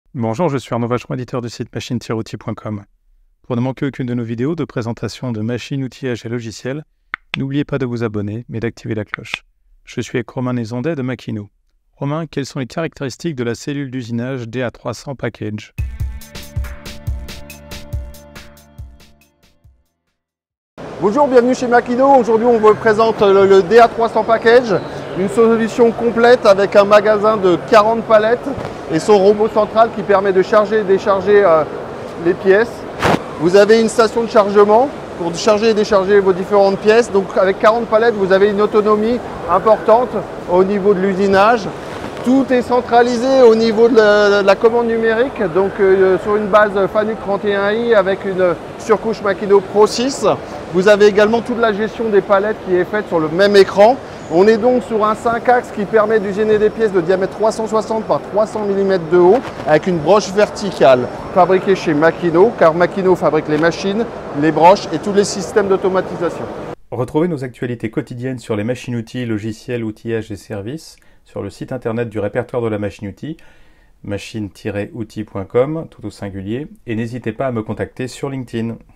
Centre d'usinage 5 axes avec son magasin automatisé à 40 palettes Makino DA300 Package
* Plus d'Actualités Machines, Logiciels, Outillages :
Interview de Romain NEZONDET de MAKINO.

Ce centre d'usinage montrera tout son potentiel lors de l'usinage de pièces complexes. Il est livré ici dans une configuration qui lui assure une grande autonomie grâce à son magasin de 40 palettes.